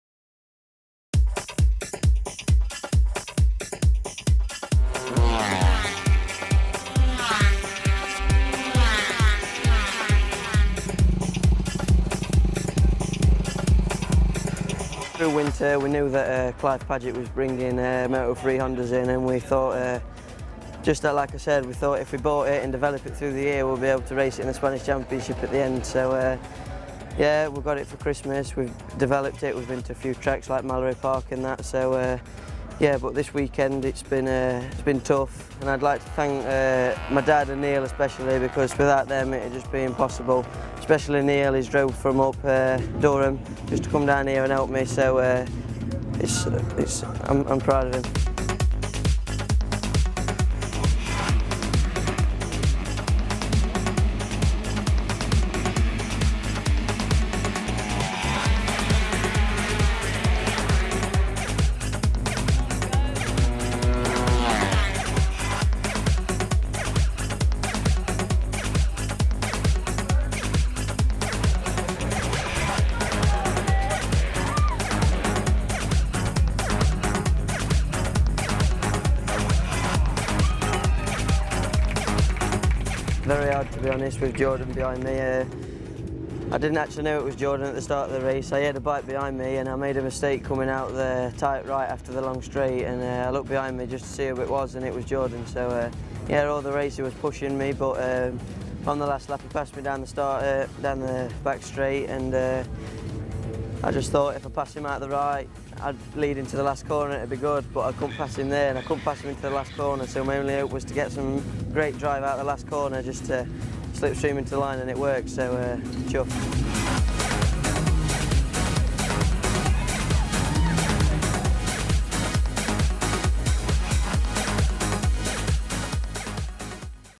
Support Class Focus: Kyle Ryde - MCE Insurance British Superbike Championship
Reigning Monster Energy Motostar British champion Kyle Ryde debuted his new Moto3 bike at Snetterton, winning at the first attempt and BSBTV was there to talk to him.